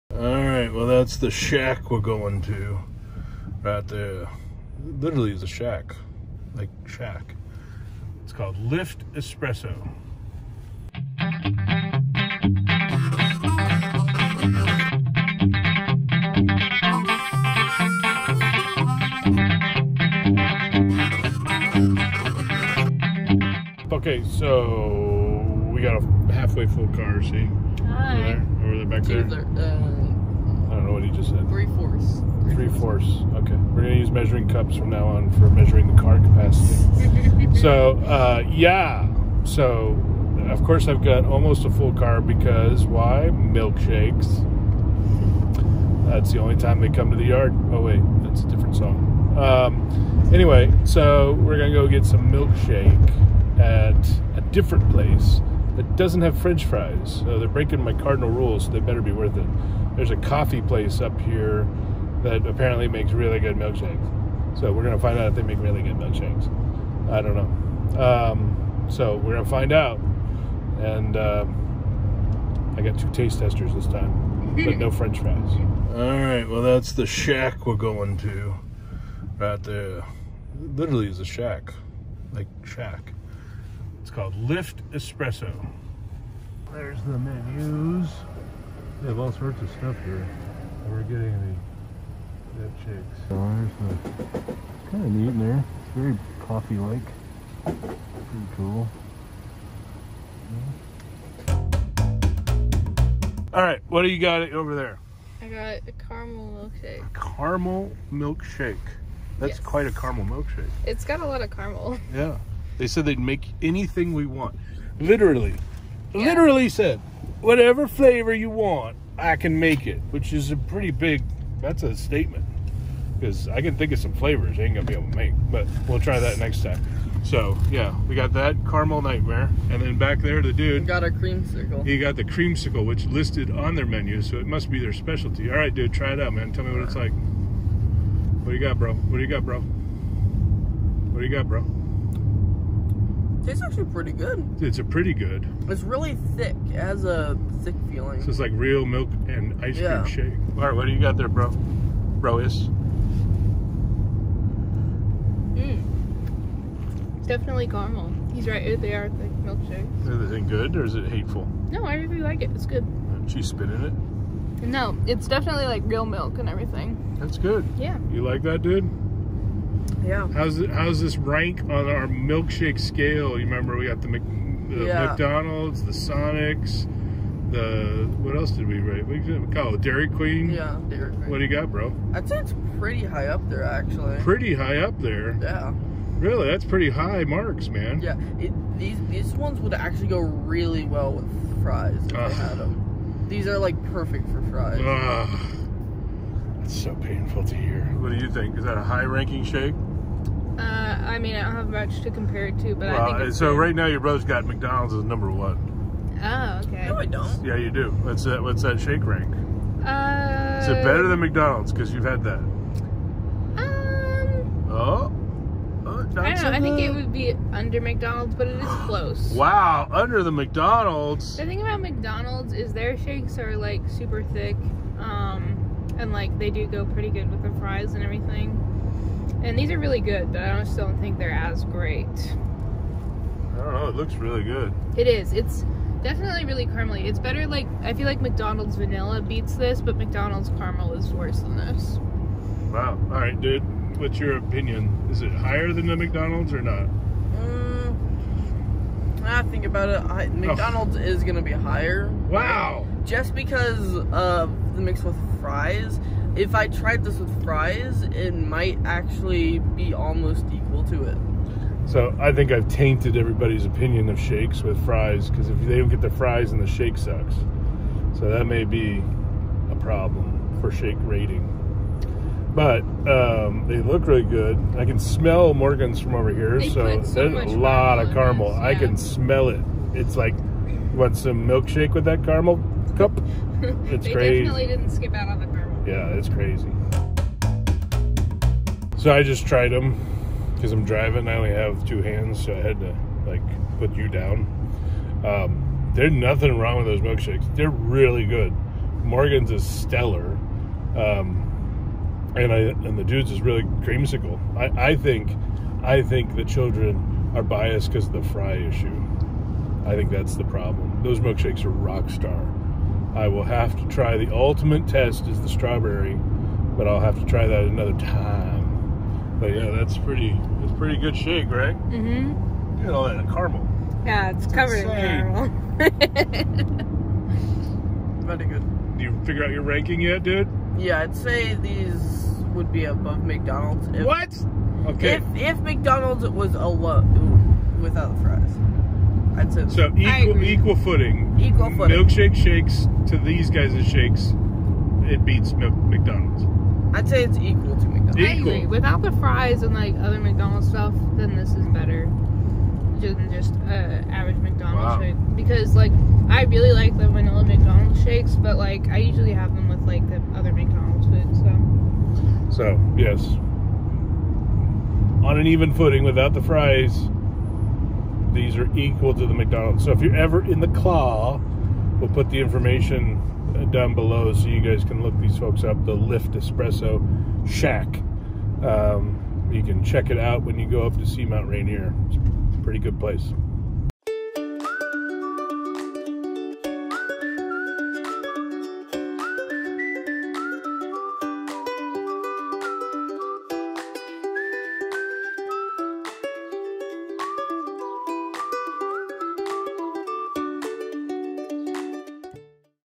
Milkshake Monday Lift Espresso - Is It The Best?
Milkshake Monday's continue. This time around the crew heads to a small place they've heard some really positive rumors about; Lift Espresso. Will it standup to the Dude's rating system?  Hard to say, can anything beat number one without frech fries to add?